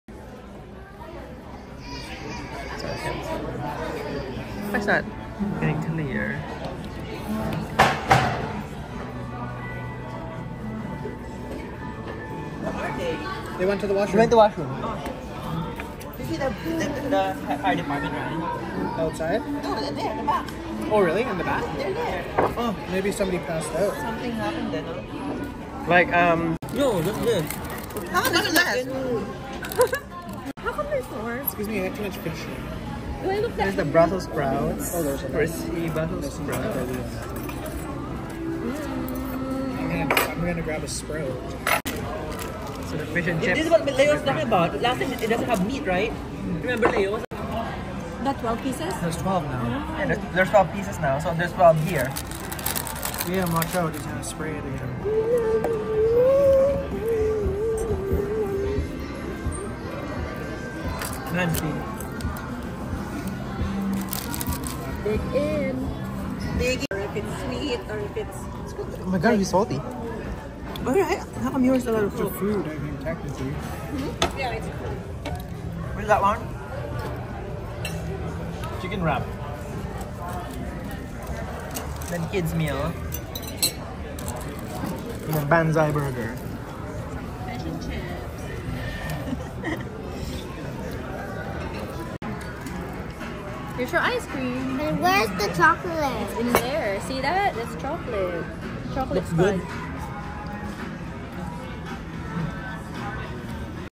Red Robin at Willowbrook Mall
Indulge in a mouthwatering dining experience at Red Robin, nestled within Willowbrook Mall! Join us as we embark on a culinary journey through a menu bursting with flavor and excitement. From sizzling burgers piled high with toppings to crispy appetizers and refreshing beverages, every bite is a celebration of American comfort food at its finest. Sit back, relax, and soak in the vibrant atmosphere as we savor each delectable dish, surrounded by the buzz of shopping and the warm hospitality of Red Robin's friendly staff. Whether you're refueling after a day of retail therapy or simply craving a delicious meal with friends and family, Red Robin at Willowbrook Mall promises to satisfy your cravings and leave you craving more. So come hungry and leave happy – because at Red Robin, every meal is a celebration! 🍔🥗🍟